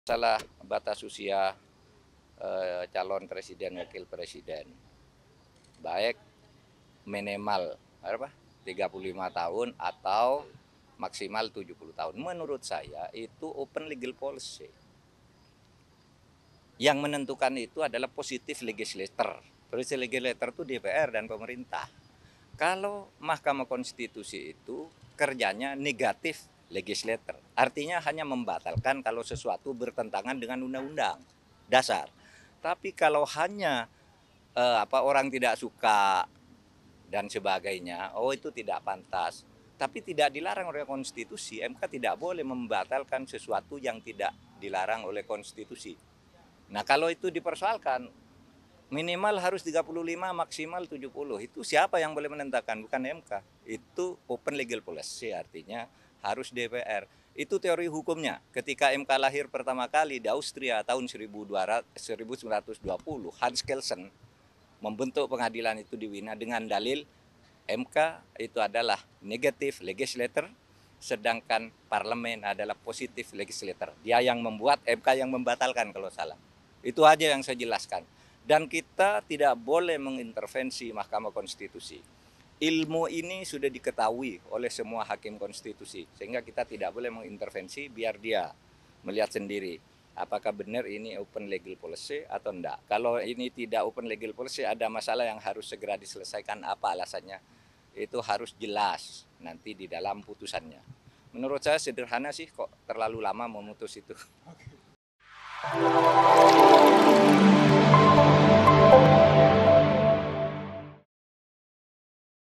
TEGAS, Mahfud Kritik Lambatnya MK soal Aturan Batas Usia Capres Cawapres
TRIBUN-MEDAN.COM- Menkopolhukam Mahfud MD mempertanyakan lambatnya proses uji materi terkait batas usia calon presiden dan calon wakil presiden di Mahkamah Konstitusi.

Menurut Mahfud, hakim konstitusi yang memahami ilmu hukum tentu mengerti bahwa keputusan usia capres-cawapres itu milik kewenangan DPR RI.

Ia mengatakansoal batas usia calon presiden dan wakil presiden, baik minimal 35 tahun atau maksimal 70 tahun, yang menetukan adalah positif legislator.

Legislator itu kata Mahfud adalah DPR dan pemerintah.